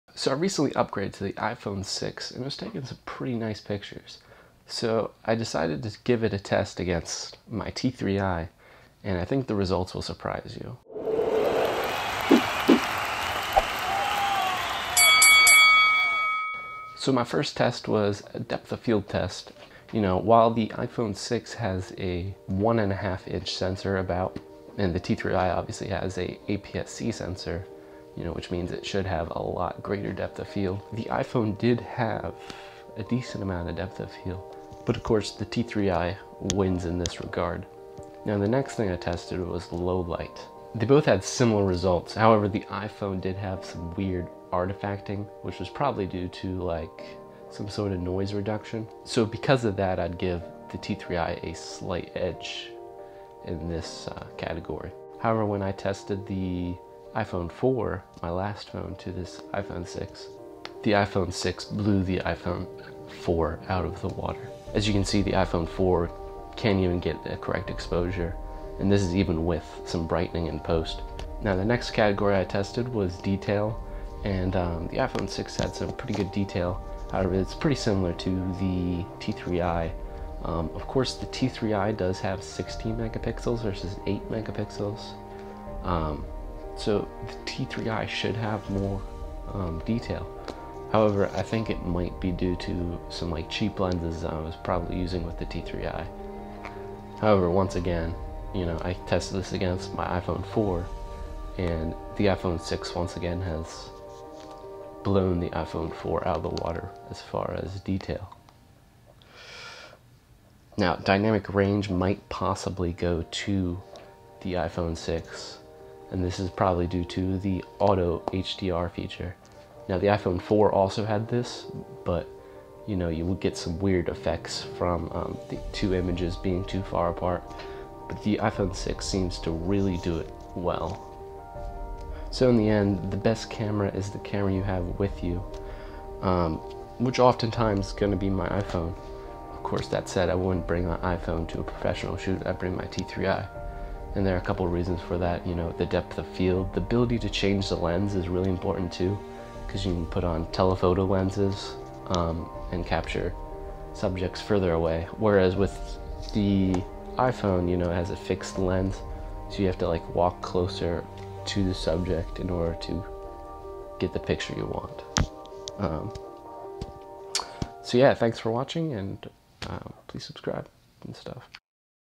iPhone 6 vs T3i Photo Shootout
Check out this video shootout between the iPhone 6 and 600D as well as some appearances of the iPhone 4.